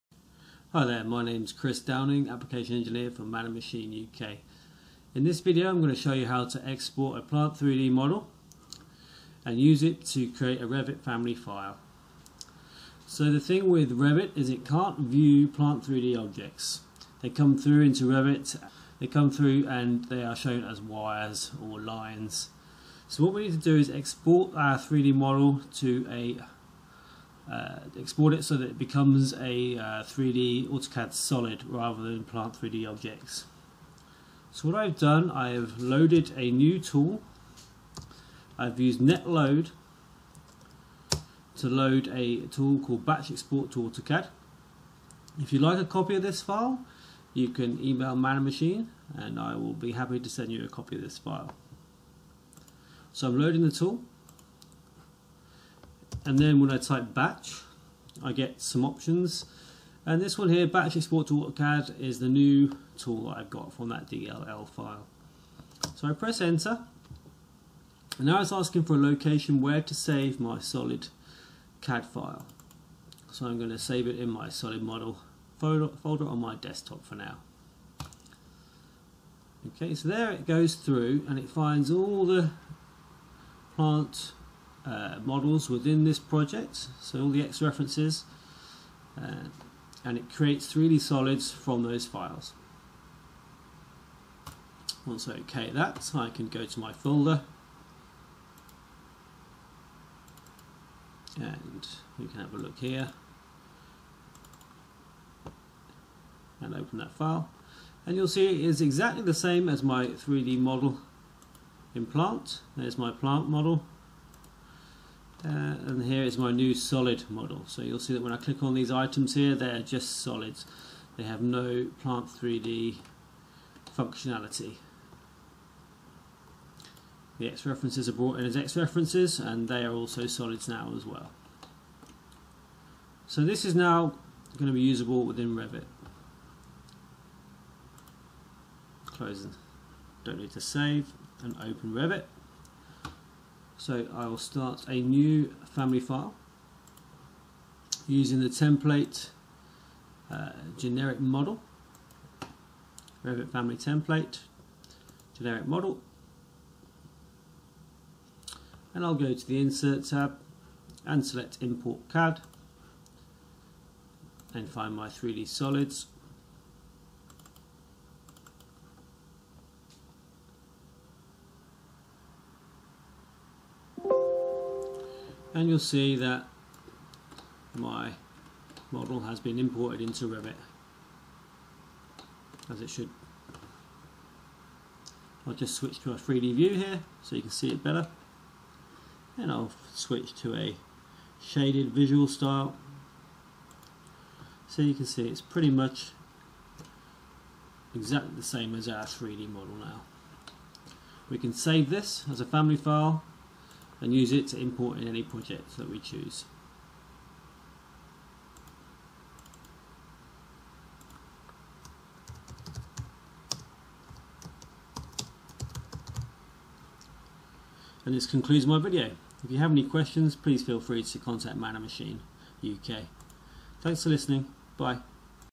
Export from Plant 3D to Import and create Revit Family File
This video shows you how you can use the ‘BATCHEXPORTTOAUTOCAD” tool to export a Plant 3D model so that it can be used to create a Revit family file, this in turn can be used in Revit projects. It enables interoperability between Plant 3D and Revit.